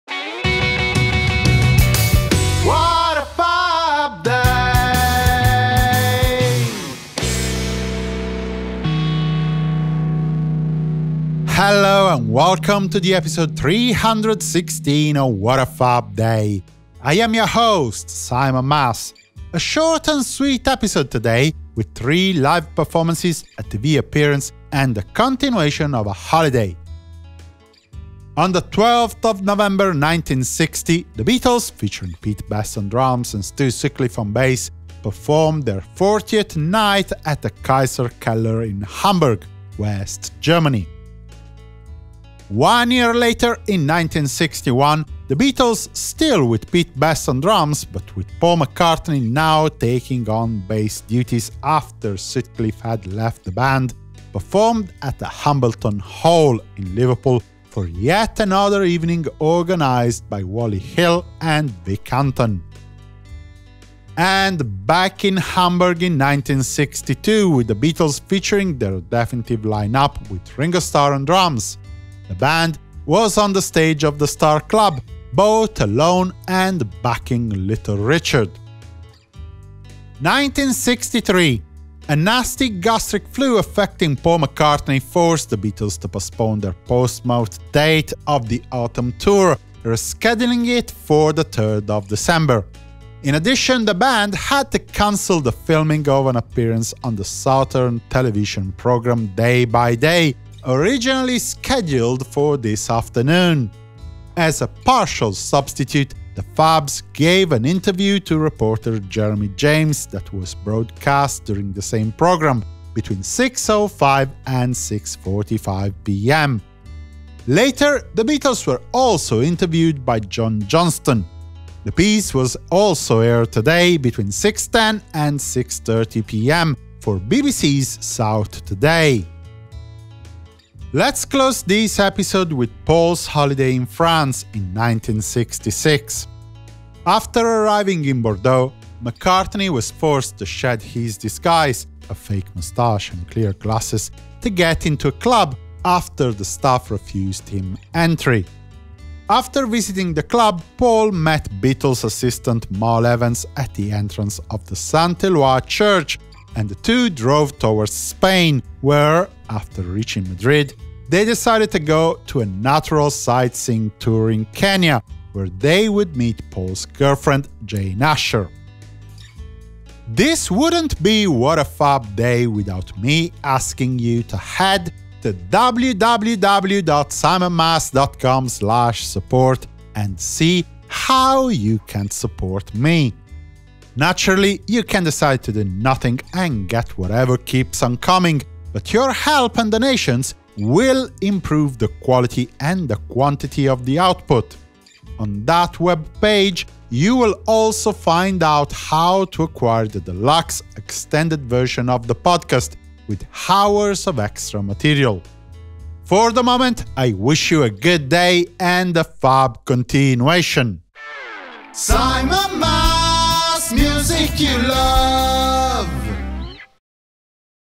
Paul McCartney 1966 French Holiday [ep. 316]
What happened in Beatles' history on the 12th of November? Listen to this podcast episode for three live performances, a TV appearance, the continuation of a holiday... and more!

***CHAPTERS***
0:00 Introduction.
0:28 In 1960, the Beatles are on the stage of the Kaiserkeller in Hamburg.
0:42 In 1961, the Beatles perform at the Hambleton Hall in Liverpool.
1:04 In 1962, the Beatles are at the Star-Club in Hamburg with Little Richard.
1:20 In 1963, the Beatles postpone a concert because of an illness.
2:10 In 1966, Paul McCartney is in France.
2:50 Conclusion.

***LINKS***
Please help me giving you even better content

Daniele Monterisi

Mariella Tarantino